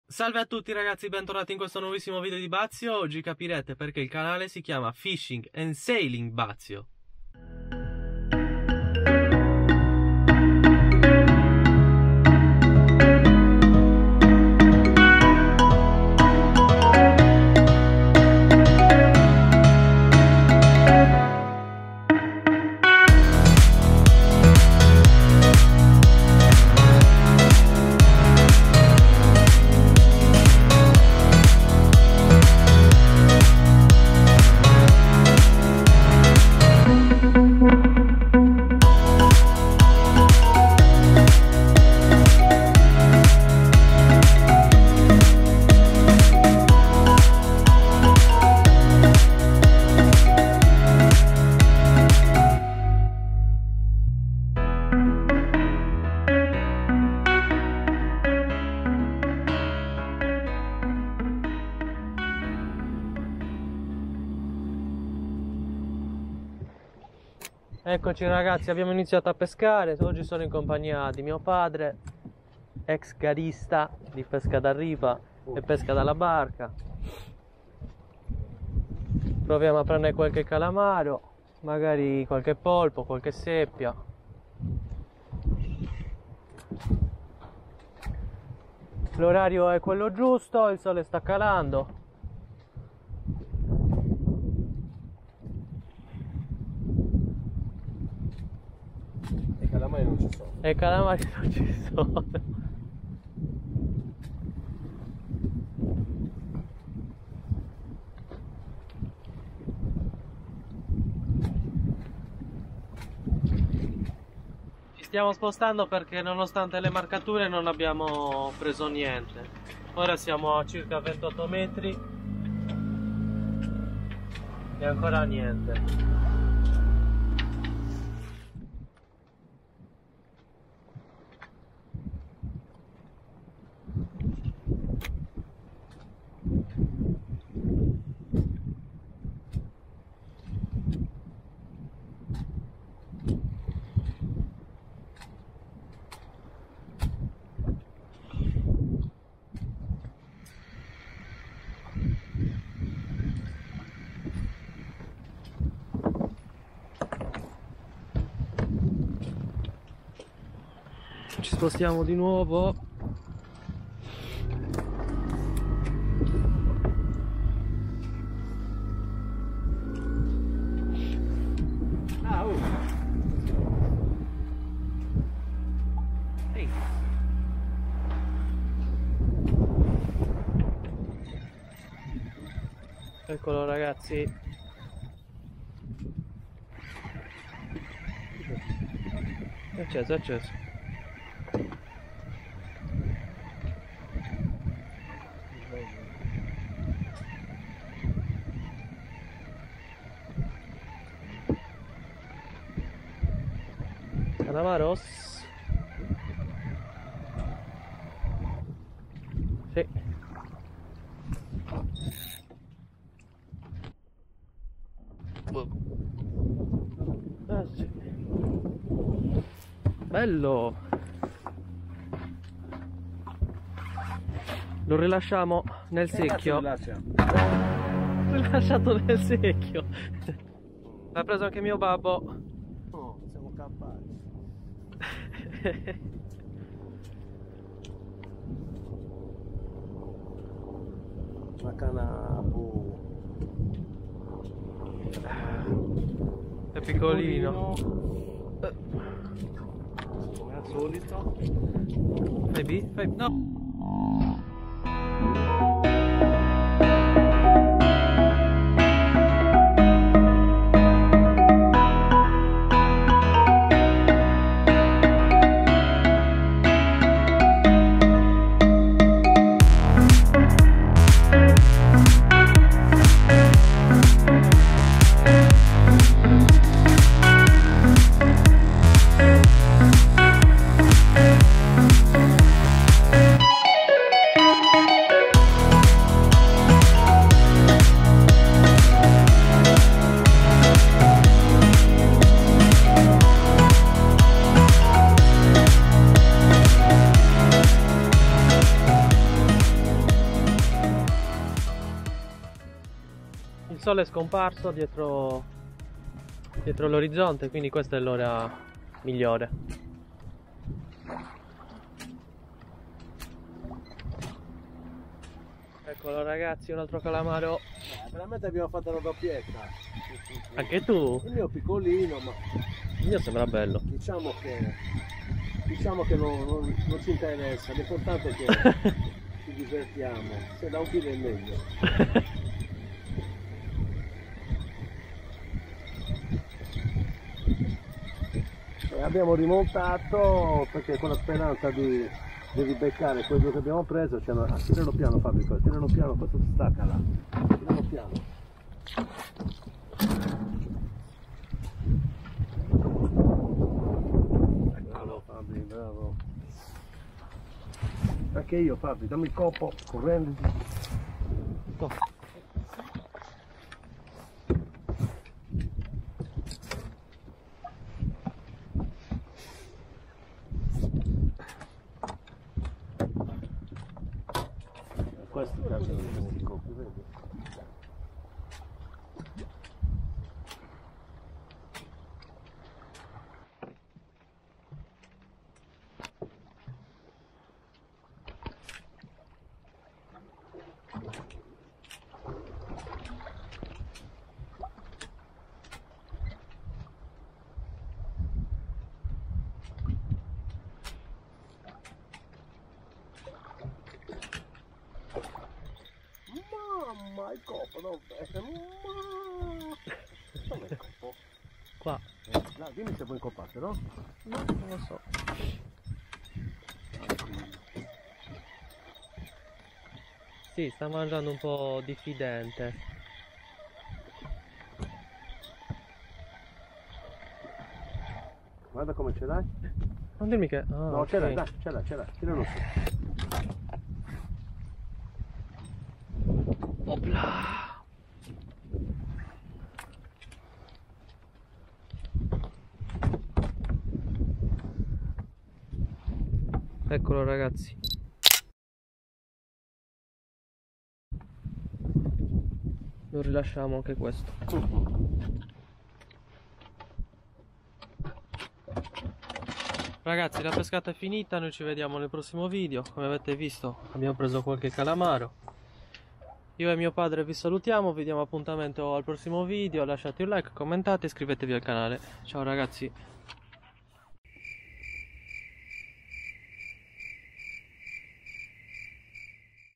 PESCA AL CALAMARO DALLA BARCA. Tataki Fishing
In questo episodio, andiamo alla ricerca di qualche calamaro dalla barca a vela con i tataki. Una pescata dedicata all'Eging dalla barca. Partiamo nel pomeriggio ma ci vorrà del tempo per catturare il primo cefalopode, che arriverà durante il tramonto.

In this Episode, we go on a Sailboat with my dad. We started the afternoon and It did take long to catch the First Squid. But at sunset they arrive.

Drop me a comment if you have any questions.

↓↓↓ATTREZZATURA UTILIZZATA↓↓↓

Canne: Nomura Kanji Egi 2.50 mt

Mulinello: Nomura Hiro Alu 4000

Trecciato: Daiwa J-Braid X8

Nylon: Asso Fluorolight 0.26

Le mie Action Cam: •GoPro Hero 2018

. Dicembre 2019.